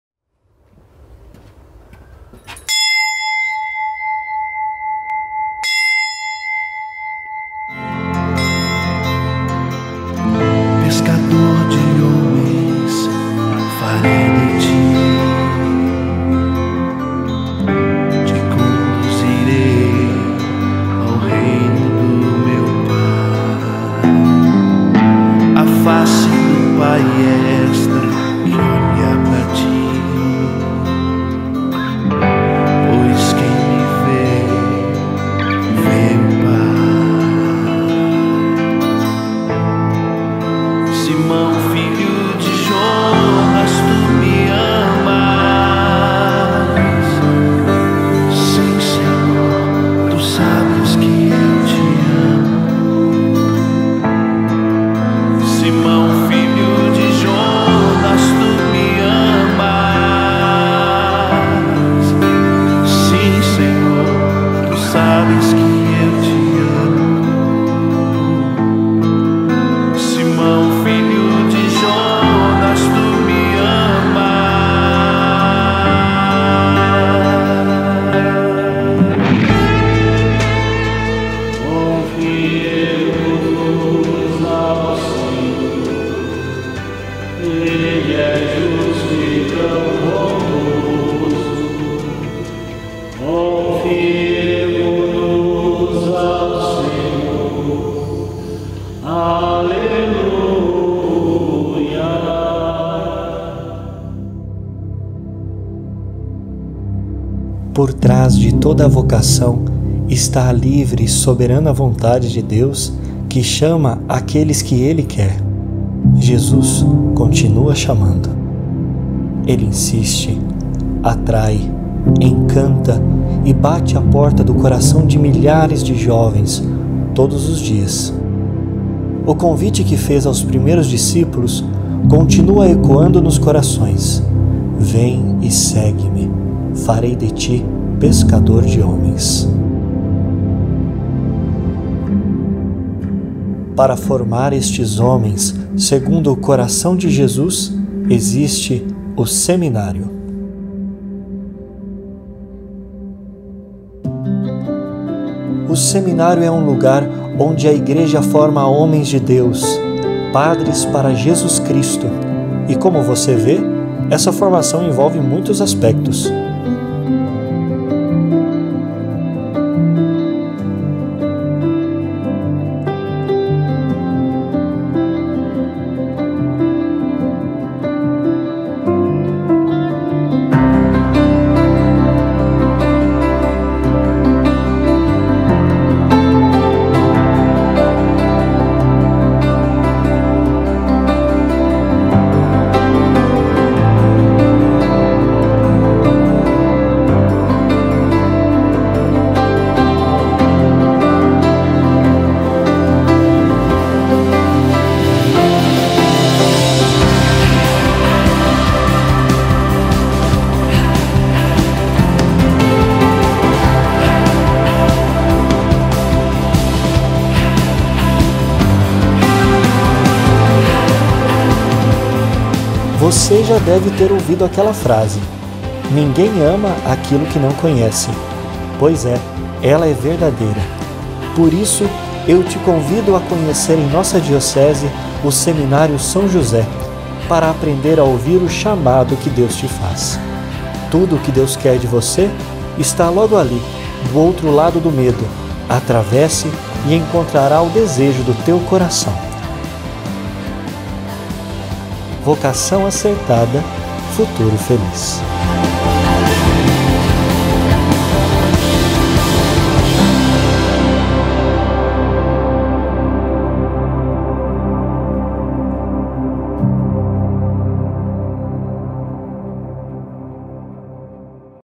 Seminário São José - Arquidiocese de Cascavel - PR
O seminário é o lugar onde a Igreja forma homens de Deus: padre para Jesus Cristo!
Conheça o Seminário São José, da Arquidiocese de Cascavel (PR).